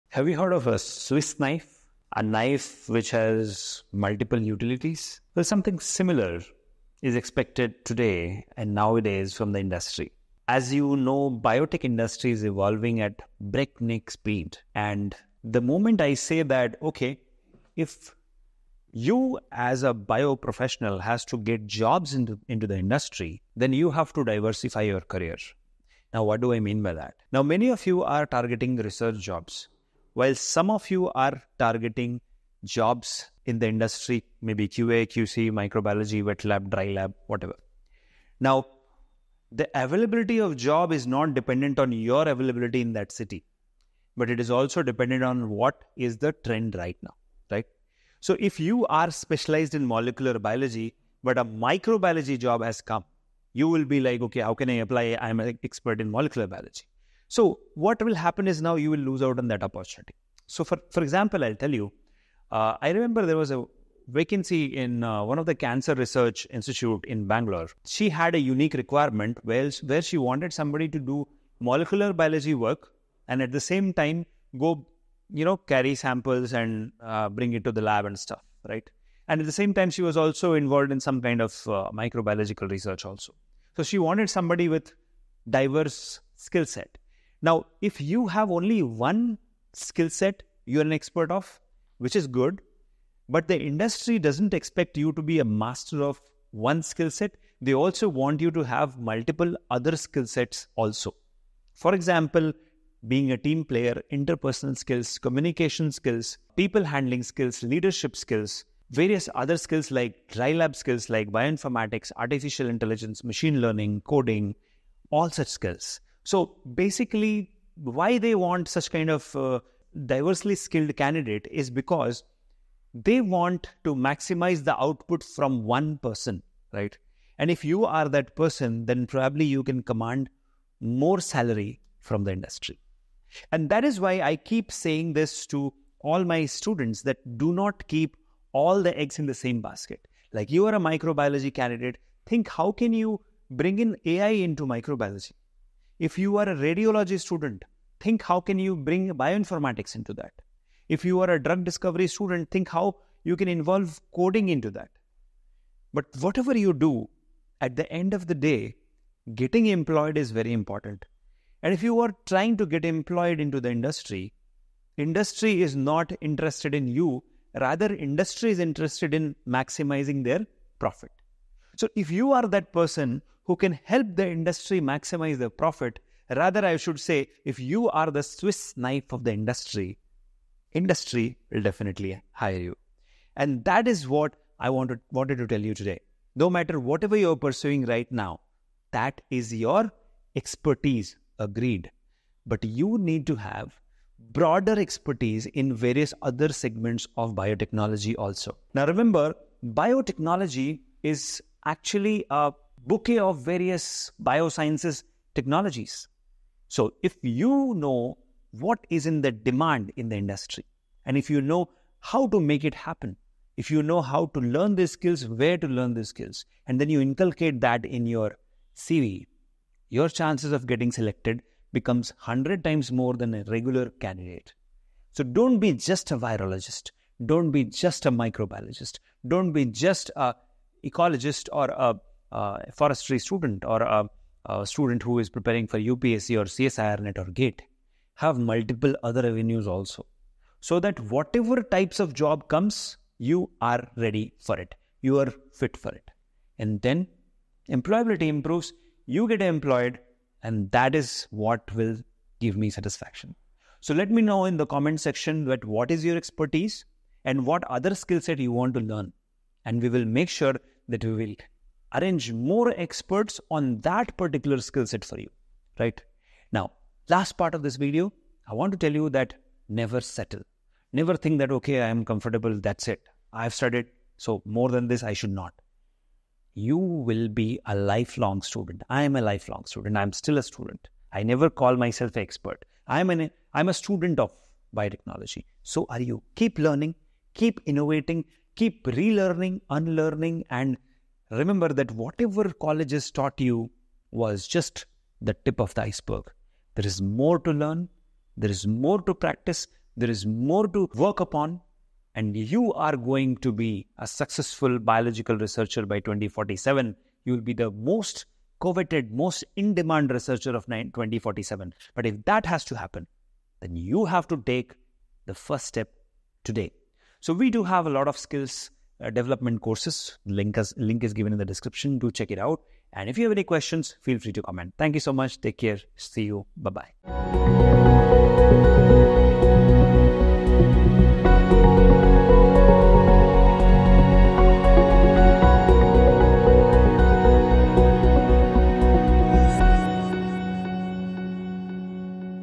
Harsh Truth! Diversify Your Biotech Career | Never Keep All Eggs in The Same Basket!
🔬 Unlock the Secrets to a Successful Biotech Career! 🔬

In this eye-opening video, we discuss the critical importance of diversifying your career in the biotechnology field. Learn why you should never rely on a single path and discover strategies to broaden your professional horizons.

💡 Key Takeaways:

- Why Diversification Matters: Understand the risks of sticking to one career path and how diversification can safeguard your future.
- Strategies for Diversification: Practical tips on how to expand your skillset, explore new opportunities, and stay adaptable in a rapidly changing industry.
- Success Stories: Real-life examples of biotech professionals who have successfully diversified their careers.
- Actionable Advice: Steps you can take today to start diversifying and enhancing your career prospects.

📈 Who Should Watch This Video:

- Biotech students and graduates
- Professionals looking to advance their biotech careers
- Anyone interested in the biotechnology industry

👉 Watch Now